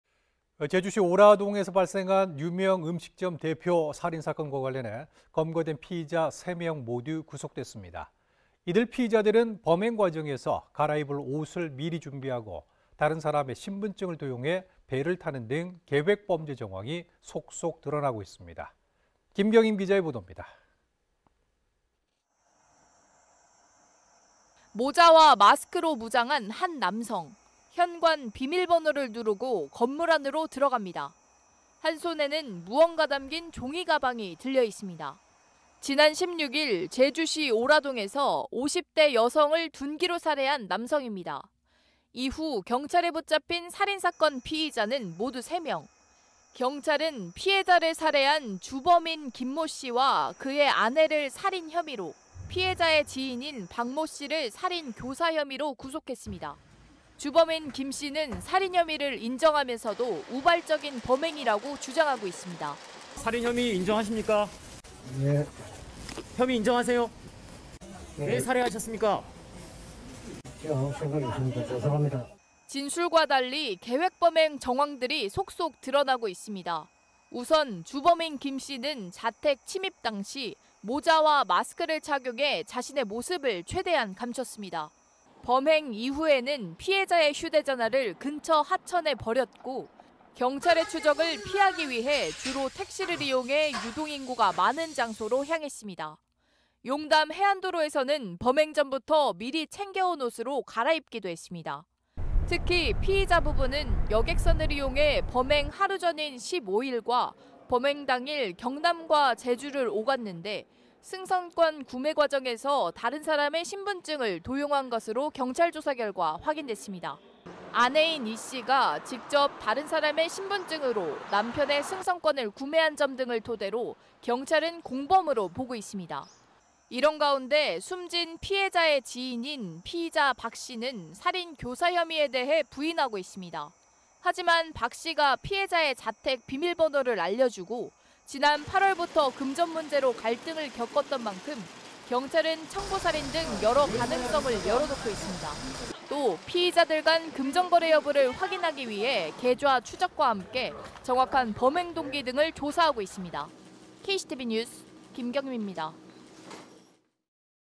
3명 모두 구속…신분증 도용에 갈아입을 옷까지
제주시 오라동에서 발생한 유명 음식점 대표 살인 사건과 관련해 검거된 피의자 3명 모두 구속됐습니다.

이런 가운데 피의자들이 범행 과정에서 갈아입을 옷을 미리 준비하고,
다른 사람의 신분증을 도용해 배를 타는 등 계획 범죄 정황이 속속 드러나고 있습니다.